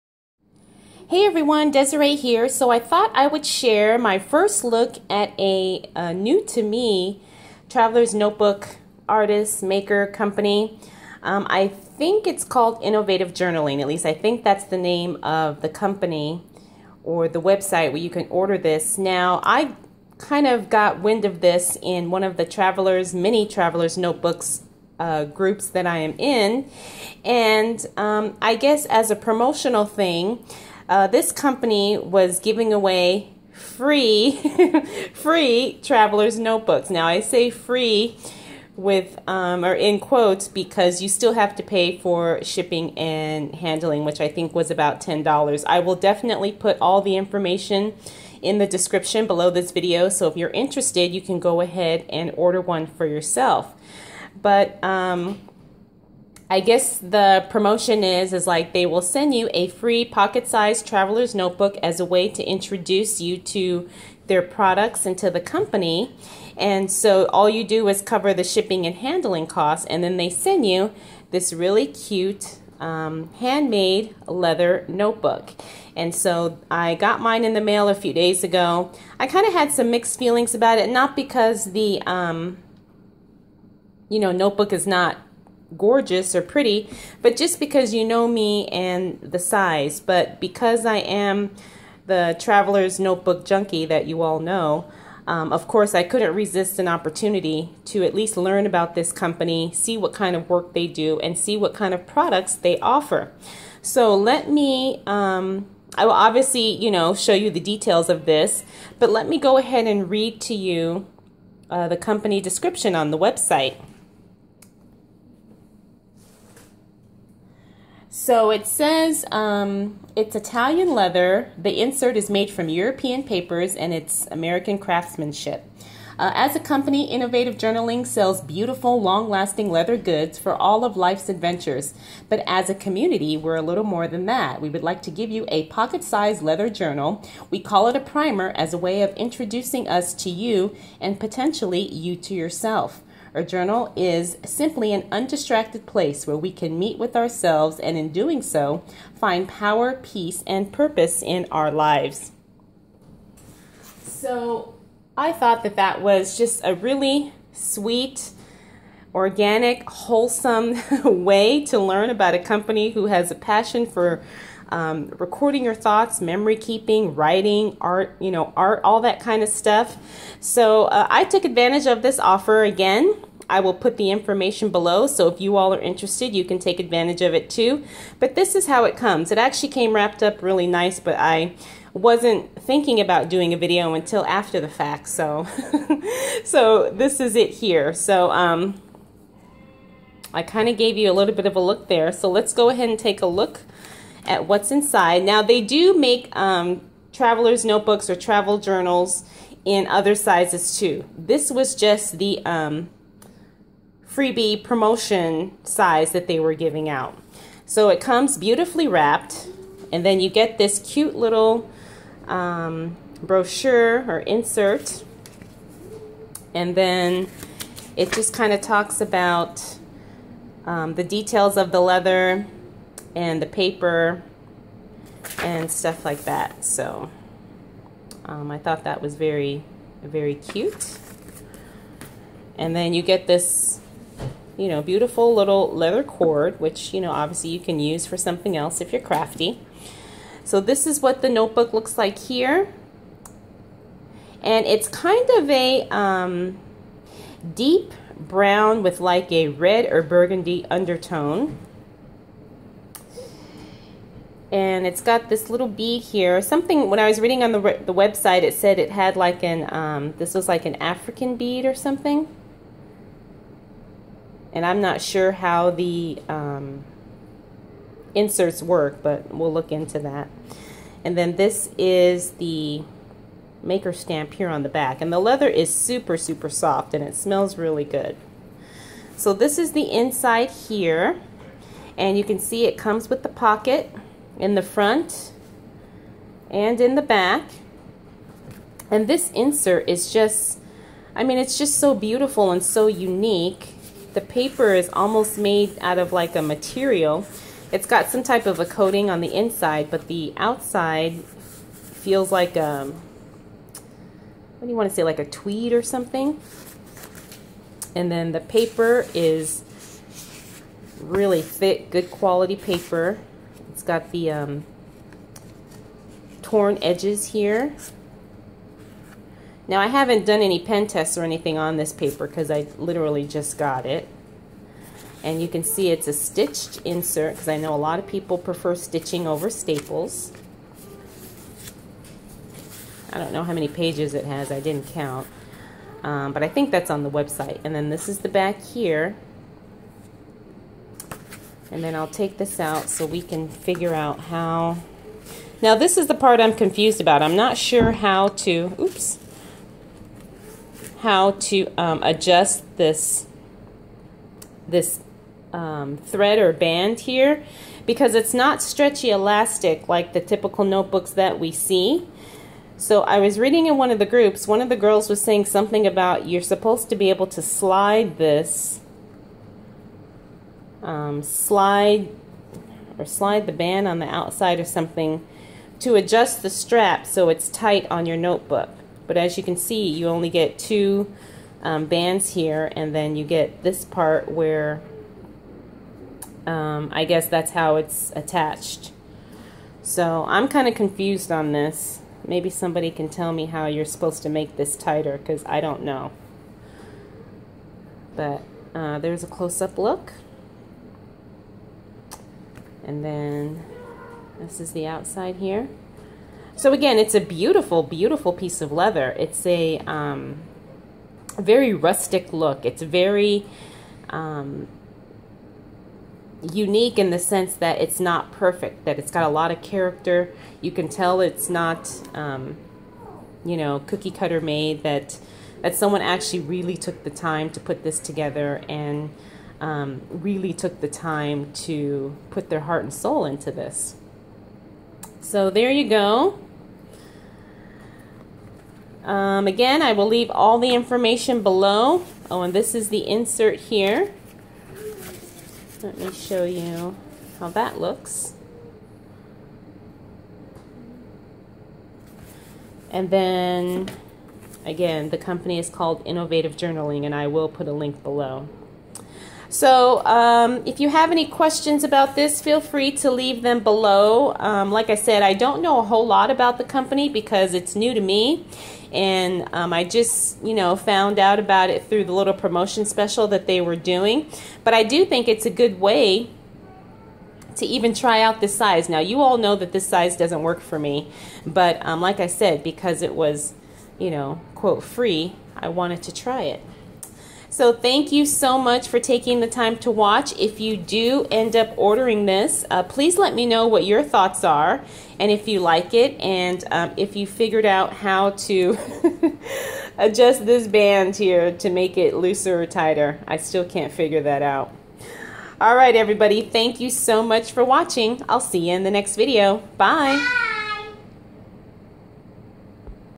First Look: Innovative Journaling - Get a 'FREE' traveler's notebook!
Sharing my first impressions of a (new to me) leather notebook from Innovative Journaling. They are currently offering a special right now that allows you to see their work firsthand. You can get a "free" pocket sized leather notebook that includes one insert, however, you do have to pay the shipping costs.